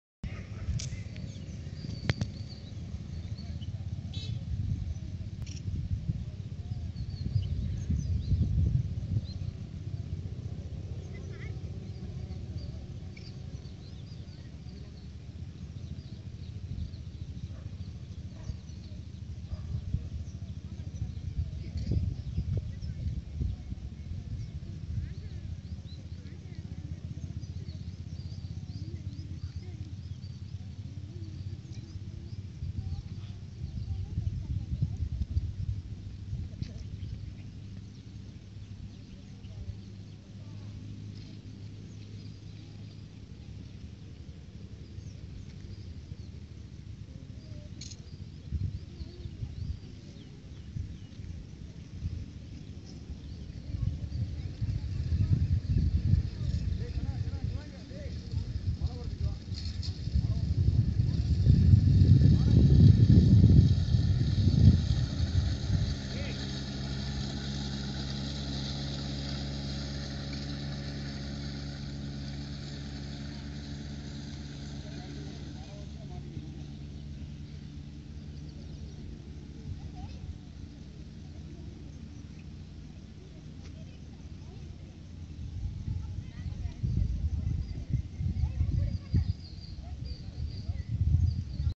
Bankers Colony housing plots are 2km from sathy road, by cooperative housing society
our hindustan cooperative housing society's plots in thelungupalayam sathy Road,, Annur, coimbatore